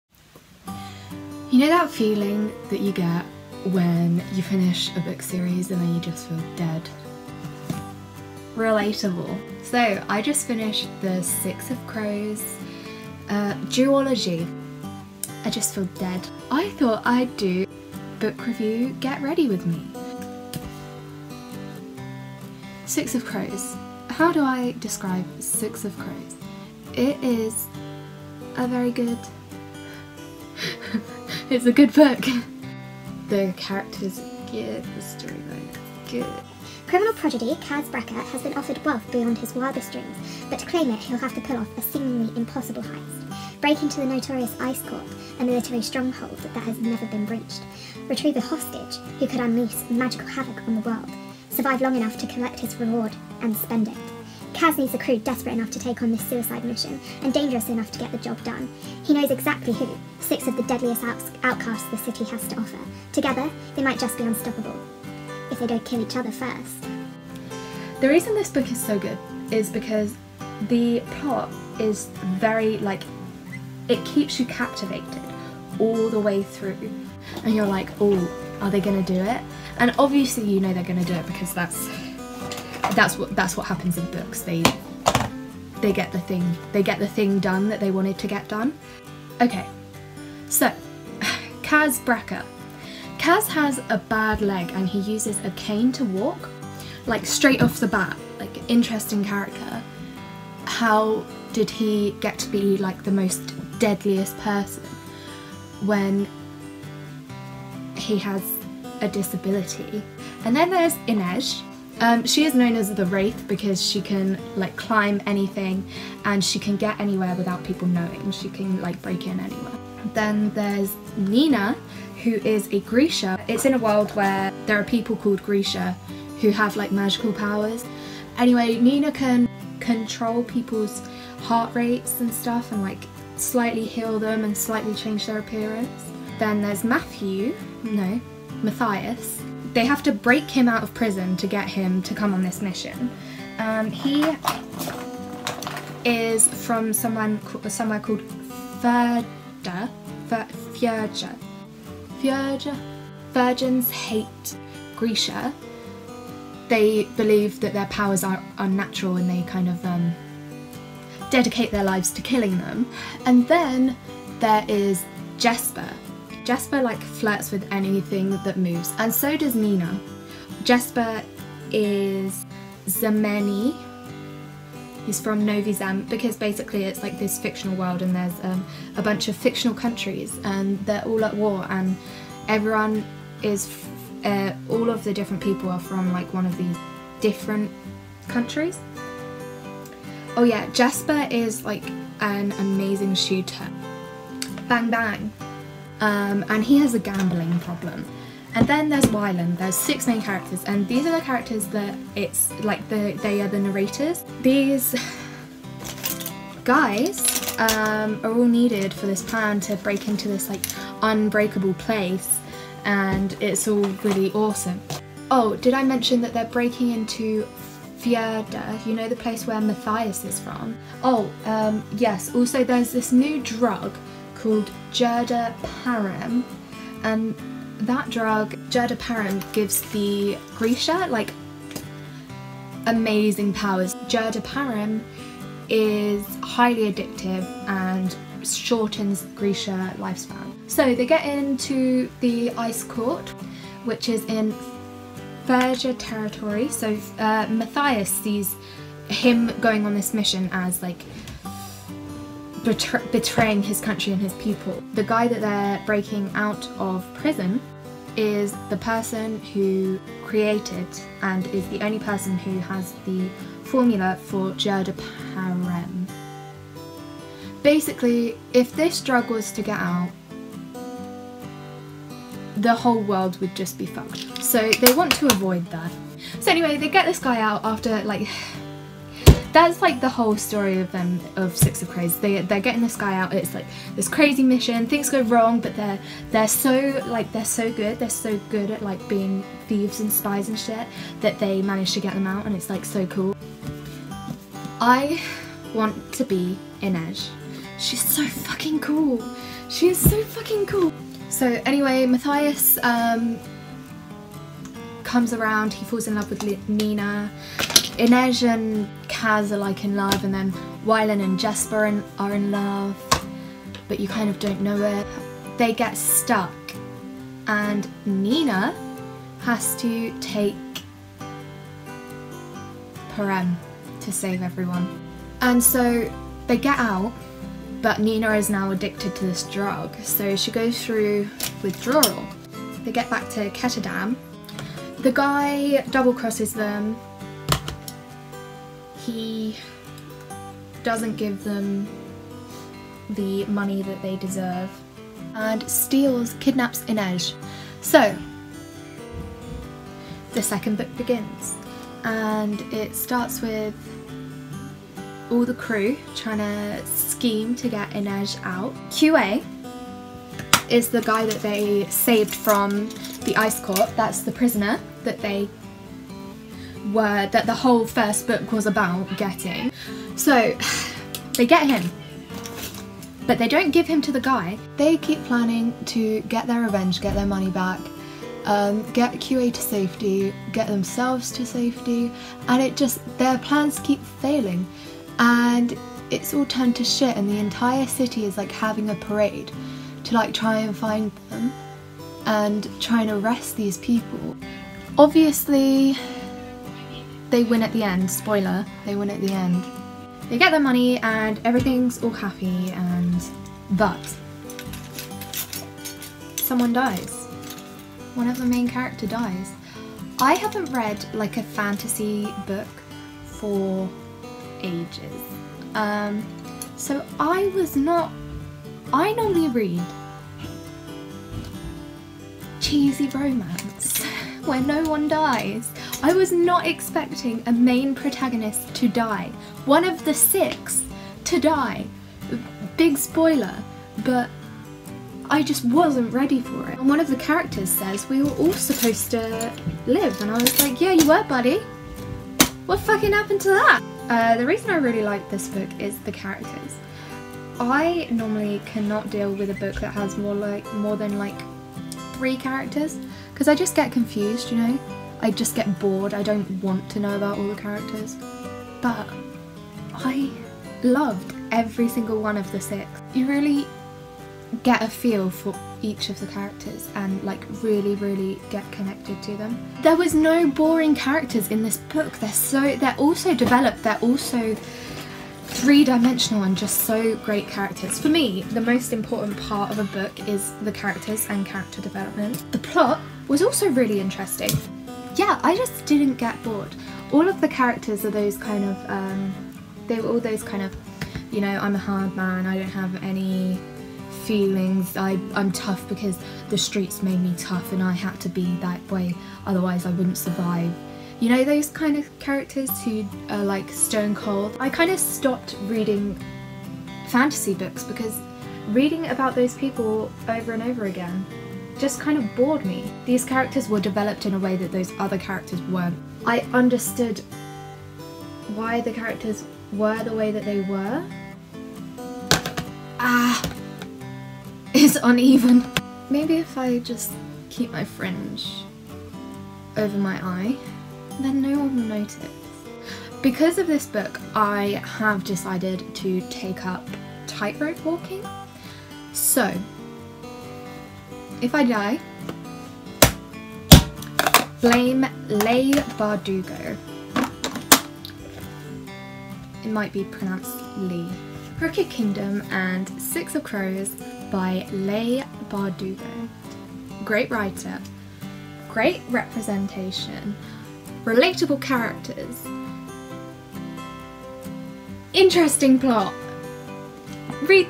Book Review GRWM: Six Of Crows
A book review of Six of Crows and Crooked Kingdom by Leigh Bardugo. These books were so good so I thought I’d talk about them.